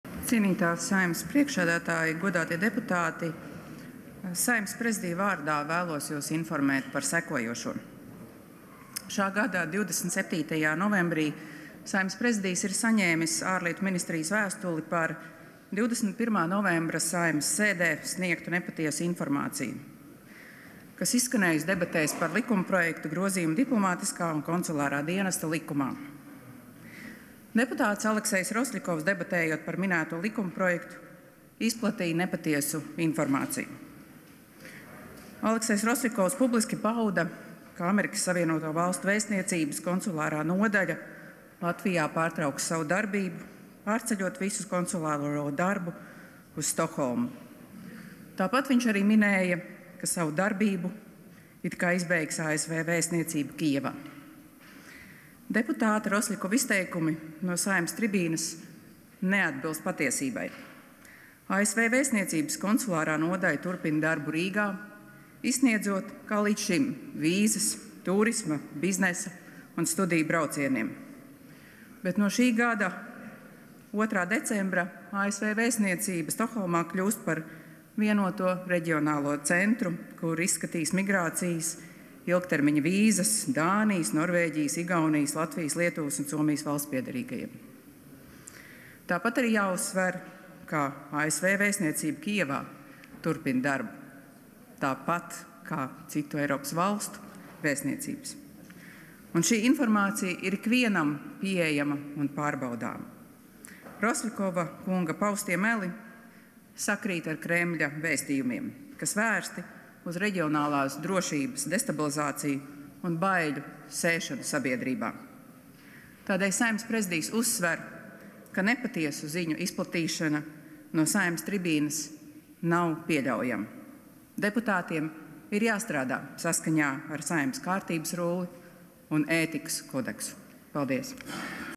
Kalniņa-Lukaševica: Nepatiesu ziņu izplatīšana no Saeimas tribīnes nav pieļaujama
Saeimas Prezidija vārdā ZANDA KALNIŅA-LUKAŠEVICA komentē 21. novembra Saeimas sēdē notikušās debates par likumprojektu “Grozījumi Diplomātiskā un konsulārā dienesta likumā”, kuru ietvarā opozīcijas deputāts Aleksejs Rosļikovs sniedzis nepatiesu informāciju par ASV vēstniecības konsulārās nodaļas Latvijā un ASV vēstniecības Kijivā darbības pārtraukšanu.